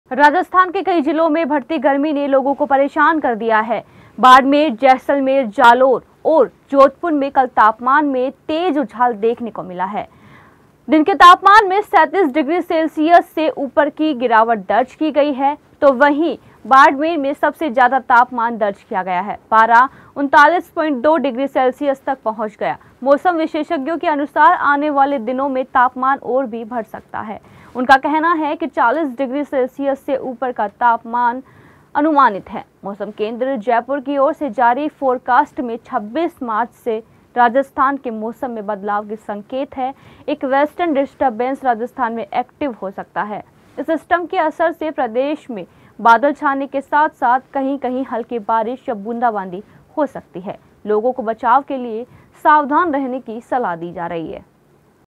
गर्मी से लोग हुए बेहाल
राजस्थान के कई जिलों में बढ़ती गर्मी ने लोगों को परेशान कर दिया है. बाड़मेर, जैसलमेर, जालोर और जोधपुर में कल तापमान में तेज उछाल देखने को मिला. दिन के तापमान में 37 डिग्री सेल्सियस से ऊपर की गिरावट दर्ज की गई.

👉 Janta Darbar News Facebook :    / jdarbar.news
👉Janta Darbar News Instagram :    / jdarbar.news
👉 Janta Darbar News Twitter:    / jantadarbarnews